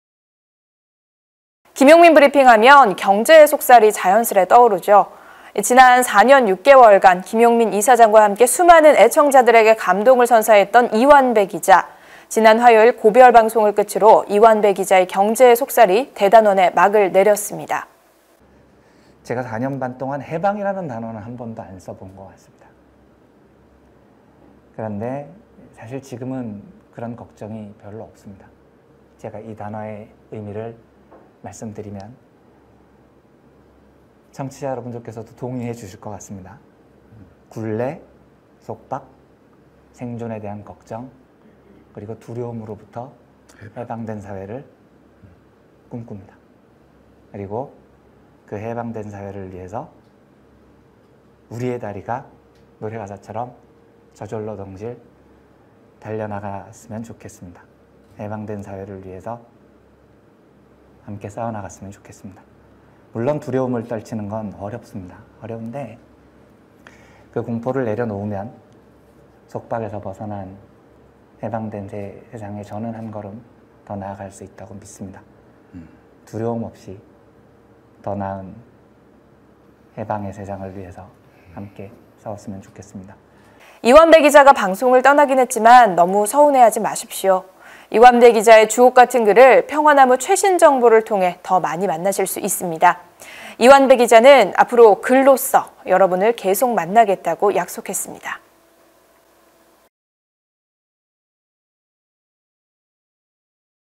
[평화나무뉴스] '이완배 기자의 경제의 속살', 4년6개월 대단원의 막 내려
2020.7.5. 평화나무뉴스

김용민 브리핑 '이완배 기자의 경제의 속살'

4년 6개월 대단원의 막 내려...

평화나무 최신정보 지면에서 계속 만나요.